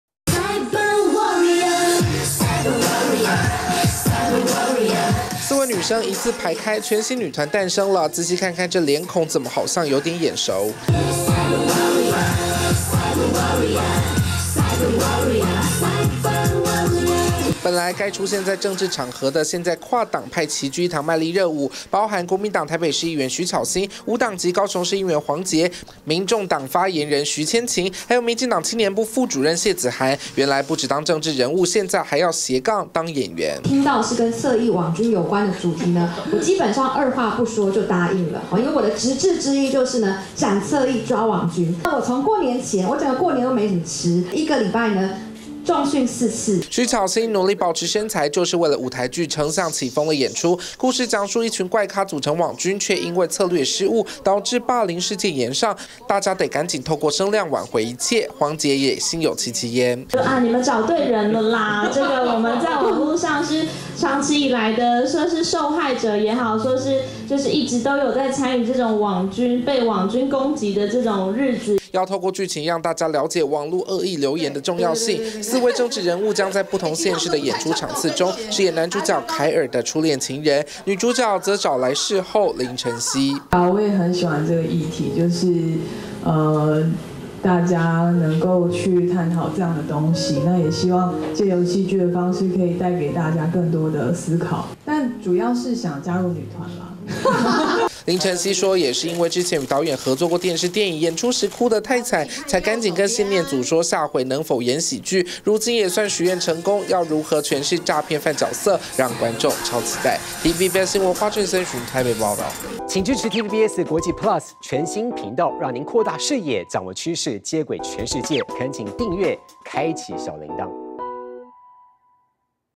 跨黨派女團成立! 徐巧芯、黃捷熱舞畫面曝...斜槓當演員「斬網軍」｜TVBS娛樂頭條@tvbsforfun
政治人物也要跟上女團風潮嗎？舞台劇《丞相，起風了》準備在六月巡迴演出，導演謝念祖找來台北市議員徐巧芯、高雄市議員黃捷等4位女性政治人物客串演出網軍的故事，今天（27號）記者會上組成跨黨派女團，跳起舞蹈，吸引觀眾目光。
丞相，起風了#徐巧芯#黃捷#謝念祖